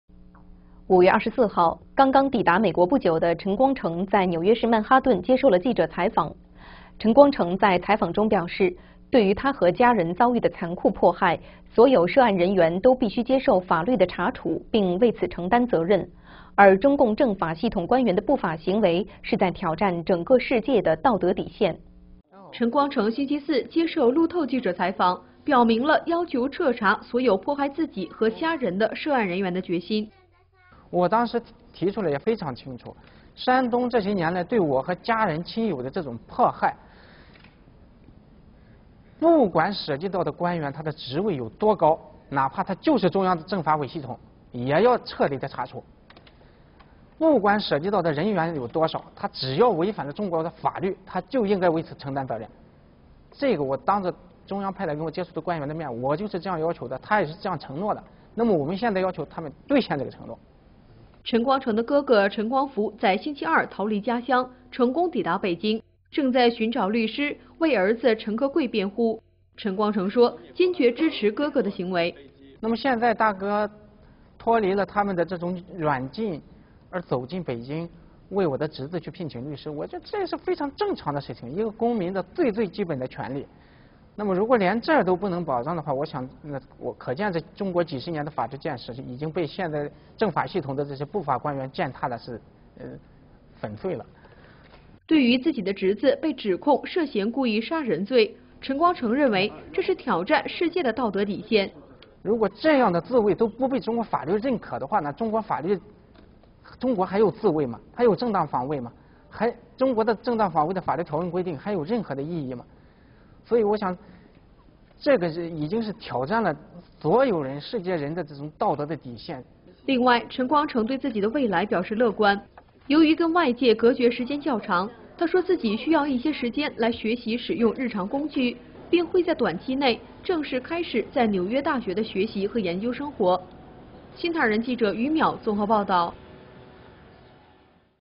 陈光诚斥责中国政法系统践踏法律
【新唐人2012年5月25日讯】5月24号（星期四），陈光诚在纽约曼哈顿接受媒体采访时表示，对于他和家人遭遇的残酷迫害，所有涉案人员都必须接受法律的查处并为此承担责任，中共政法系统官员的不法行为是在挑战整个世界的道德底线。

陈光诚星期四接受路透记者采访，表明了要求彻查所有迫害自己和家人的涉案人员的决心。

中国盲人律师陈光诚："我当时提出的非常清楚，山东（政府）这些年来对我和家人、亲友的这种迫害，不管涉及到的官员的职位有多高，哪怕就是中央的政法委系统，也要彻底地查处，不管涉及到的人员有多少。只要违反了中国的法律，他就应该为此承担责任。这个我当着中央派来和我接触的官员的面我就是这样要求的，他也是这样承诺的，那么我们现在要求他们兑现这个承诺。"

陈光诚的哥哥陈光福在星期二逃离家乡，成功抵达北京，正在寻找律师为儿子陈克贵辩护。陈光诚说，坚决支持哥哥的行为。

陈光诚："那么现在大哥脱离了他们的这种软禁而走进北京，为我的侄子聘请律师，我觉得这是最正常的事情，这是一个公民最基本的权利。那么如果连这都不能保障的话，可见中国几十年的法制建设，已经被这些政法系统的不法官员践踏得粉碎了。"

对于自己的侄子被指控"涉嫌故意杀人罪"，陈光诚认为这是挑战世界的道德底线。